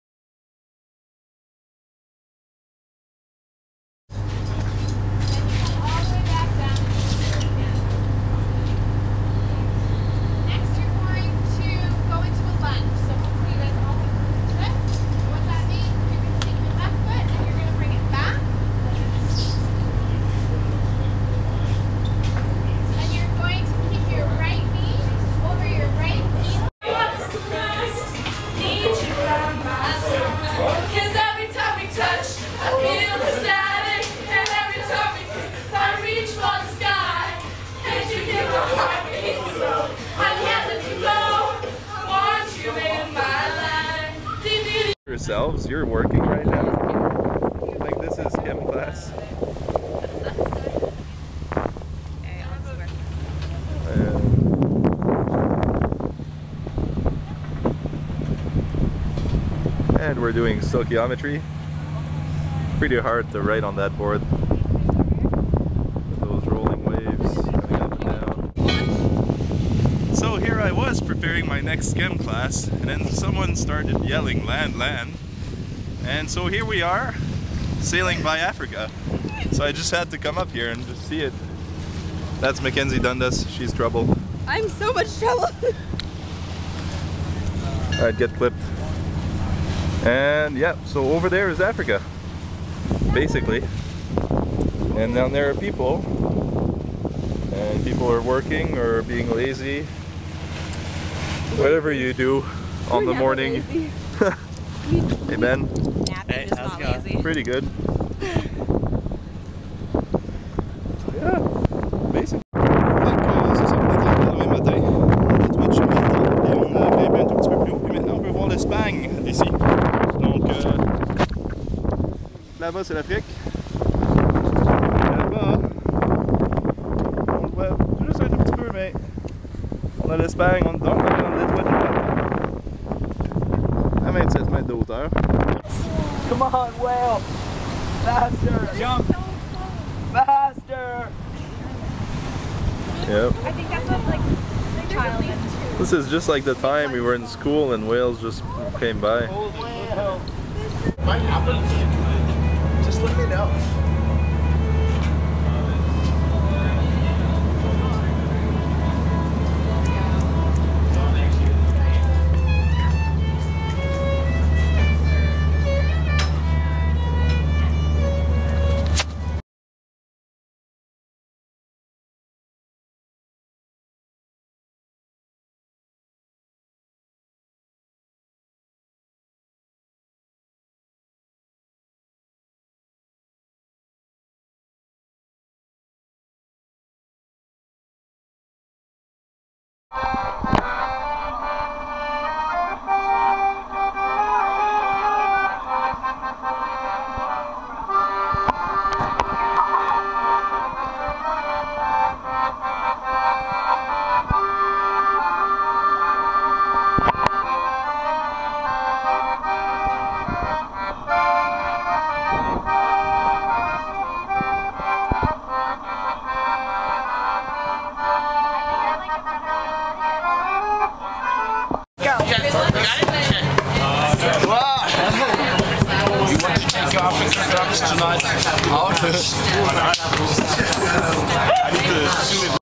journée type class afloat.m4v
Un petit essai pour montrer à quel point la vie à bord est chargée et incroyable!